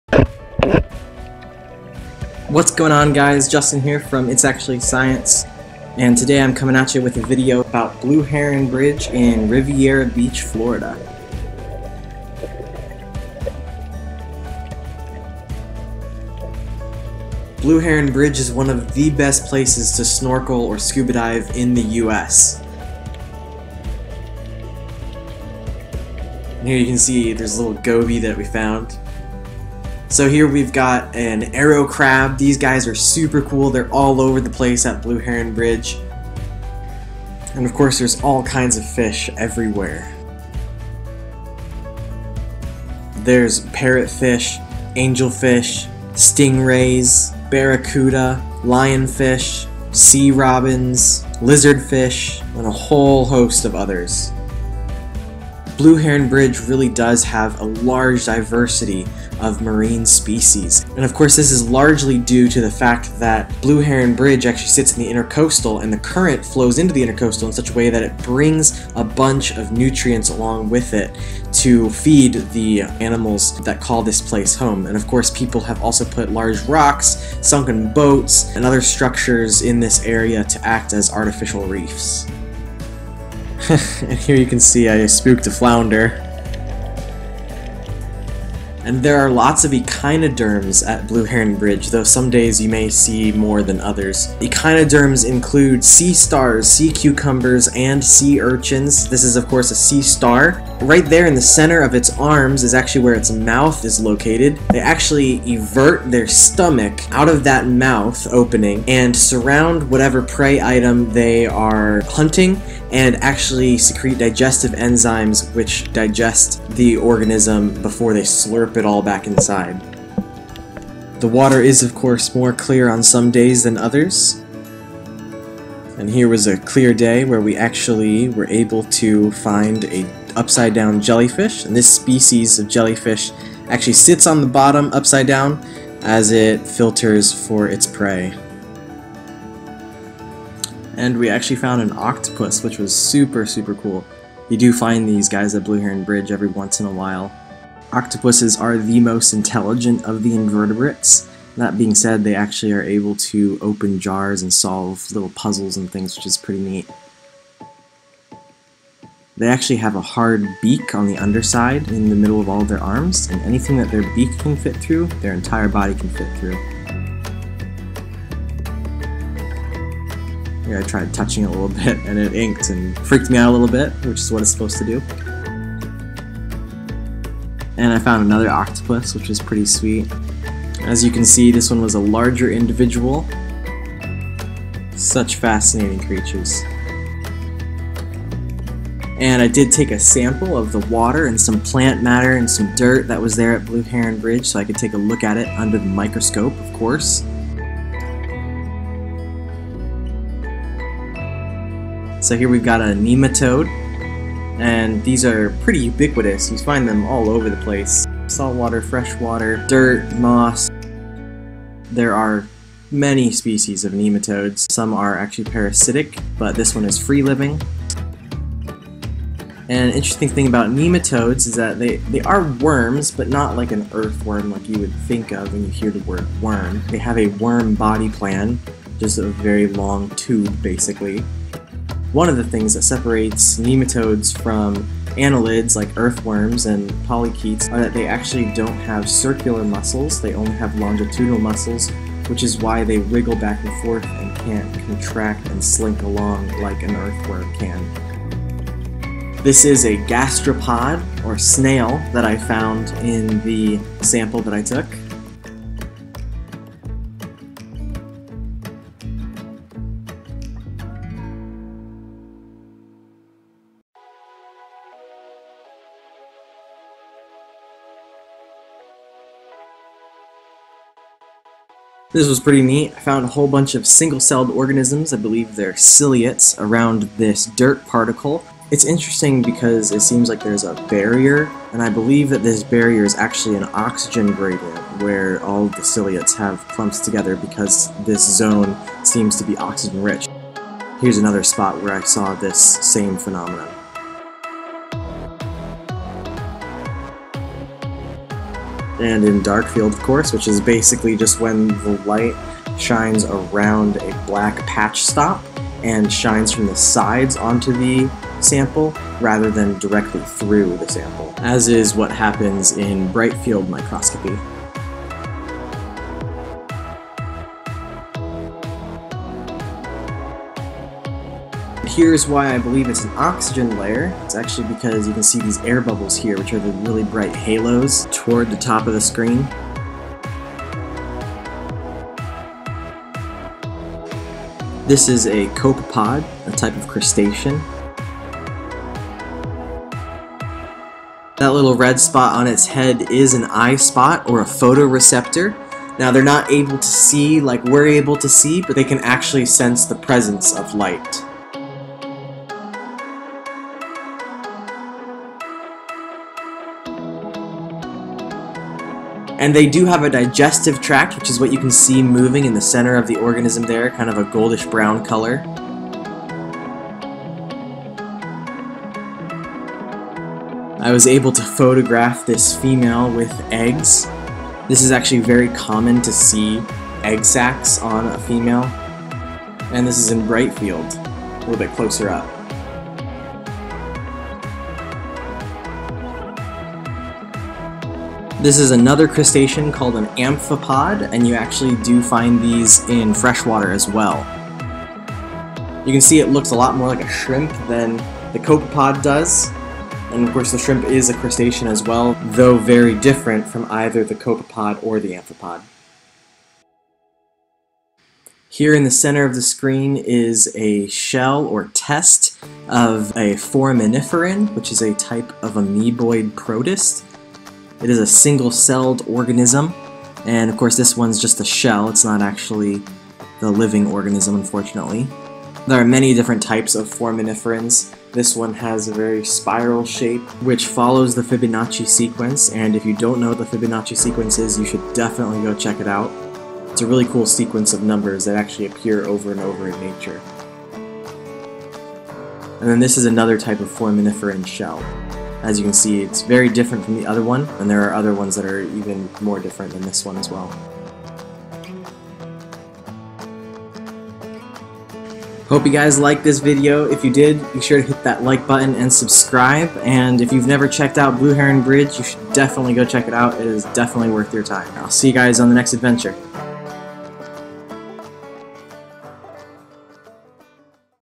Blue Heron Bridge Snorkeling Adventure!
Blue Heron Bridge is an awesome place to snorkel or scuba dive! There is a huge diversity of marine life there! I have been there several times so I wanted to make a video to give you a taste of what you might find there!